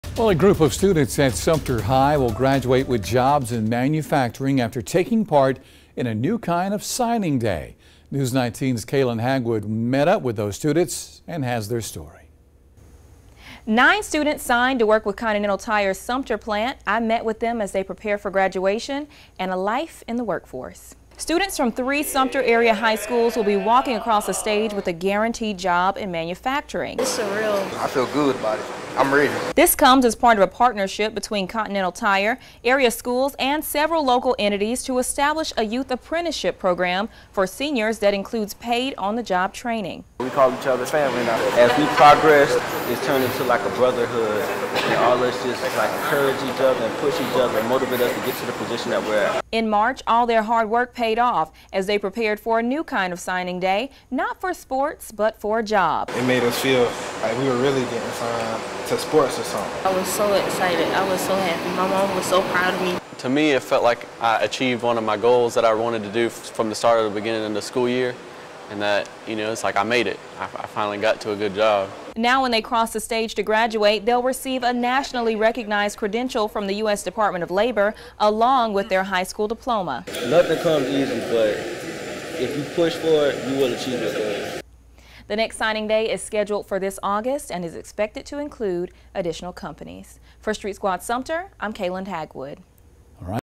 A career signing day
Students at Sumter high school will graduate with jobs in manufacturing